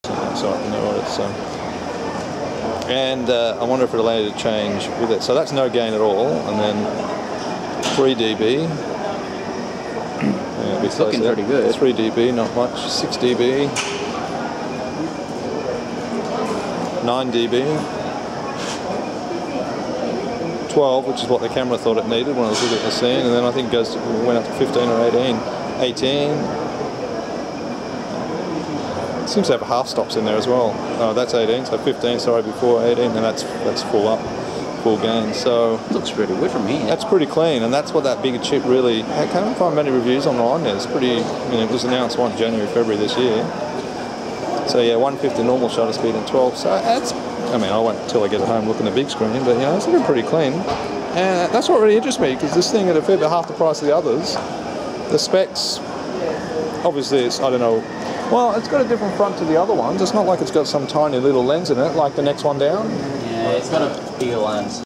Panasonic HC-V700 gain test
I was interested in how the Panasonic HC-V700 PAL camcorder with single 1/2.33" MOS sensor performed in low light - the exposure is manual stepping up from 0 dB through to 18dB, filmed using the 1080p50 mode. This 202.6MB raw MTS file was uploaded to Youtube - compression obviously at the mercy of Youtube process.